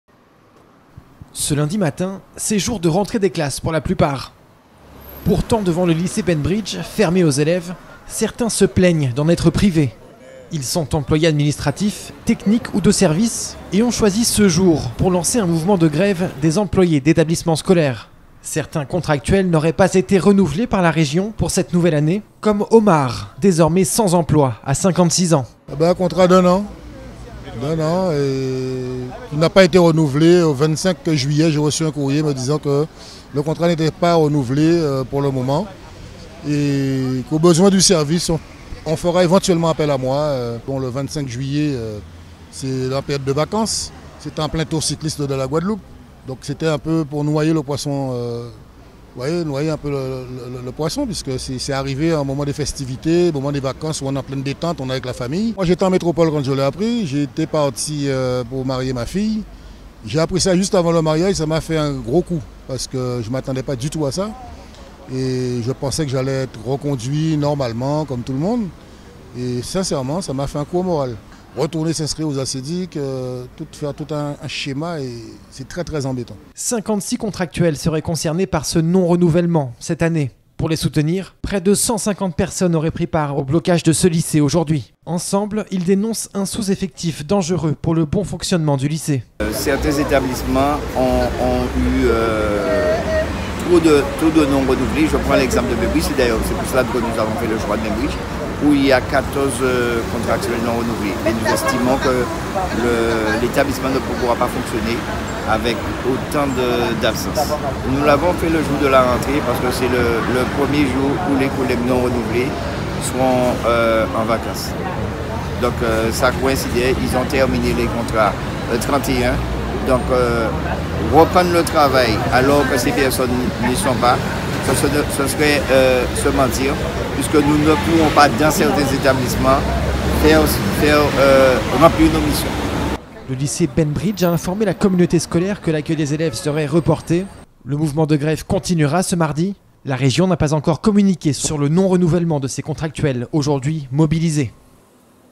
🔴 Pas de rentrée des classes aujourd'hui au lycée Baimbridge à cause d'un mouvement de grève!
"J'ai reçu un courrier le 25 juillet me disant que mon contrat n'était pas renouvelé"

Certains employés d'établissement scolaires auraient été privés de rentrée ⤵️